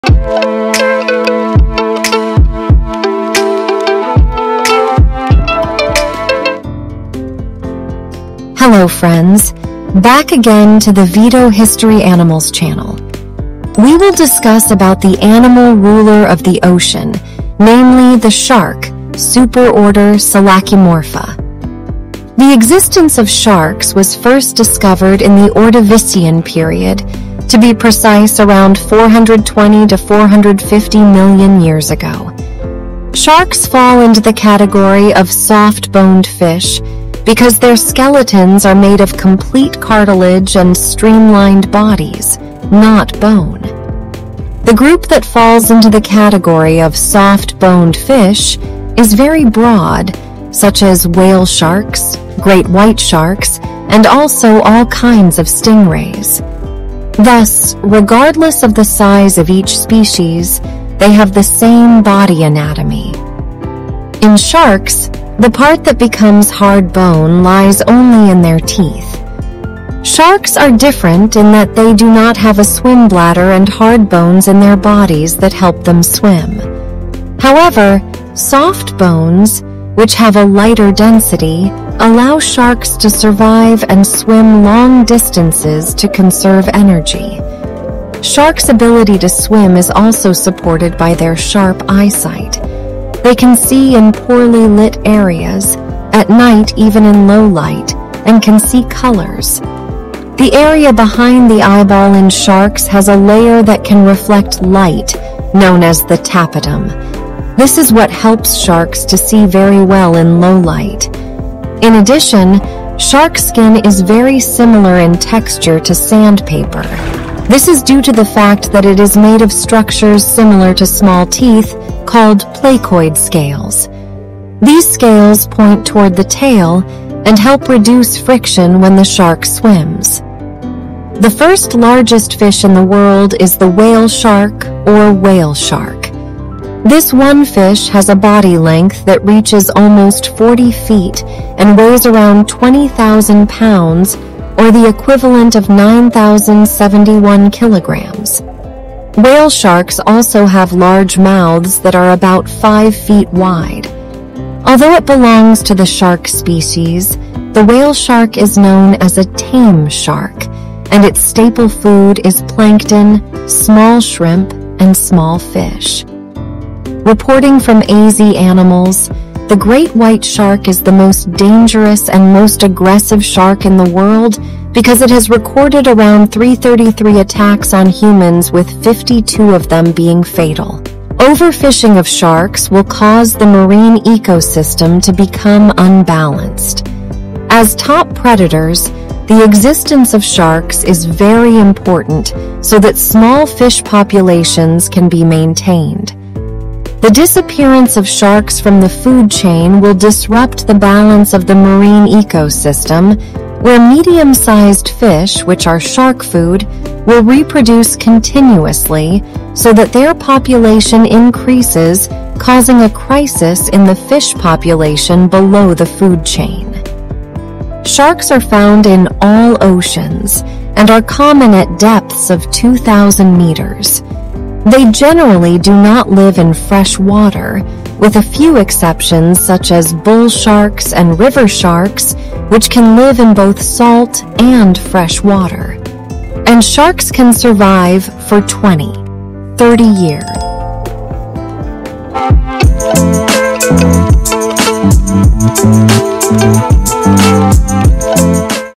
Who is the Lord of the seas?
We will discuss about the animal ruler of the Ocean, namely the Shark (Superorder Selachimorpha).
The existence of sharks was first discovered in the Ordovician period, to be precise around 420 to 450 million years ago. Sharks fall into the category of “soft-boned fish” because their skeletons are made of complete cartilage and streamlined bodies, not bone. The group that falls into the category of "soft-boned fish" is very broad, such as whale sharks, great white sharks, and also all kinds of stingrays. Thus, regardless of the size of each species, they have the same body anatomy. In sharks, the part that becomes hard bone lies only in their teeth.
sharks are different in that they do not have a swim bladder and hard bones in their bodies that help them swim. However, soft bones, which have a lighter density, allow sharks to survive and swim long distances to conserve energy.
Sharks' ability to swim is also supported by their sharp eyesight. They can see in poorly lit areas, at night even in low light, and can see colors. The area behind the eyeball in sharks has a layer that can reflect light, known as the tape tum. This is what helps sharks to see very well in low light. In addition, shark skin is very similar in texture to sand paper, this is due to the fact that it is made of structures similar to small teeth called placid scales. These scales point toward the tail and help reduce friction when the shark swims.
The first largest fish in the world is the whale shark or whale shark. This one fish has a body length that reaches almost 40 feet and weighs around 20 thousand pounds or the equivalent of 9,071 kilograms. Whale sharks also have large mouths that are about 5 feet wide.
Although it belongs to the shark species, the whale shark is known as a "tame" shark, and its staple food is plankton, small shrimp and small fish.
Reporting from AZ Animals, the great white shark is the most dangerous and most aggressive shark in the world because it has recorded around 333 attacks on humans with 52 of them being fatal.
Over fishing of sharks will cause the marine ecosystem to become unbalanced. As top predators, the existence of sharks is very important so that small fish populations can be maintained.

(source from fisheries.noaa and fascinating)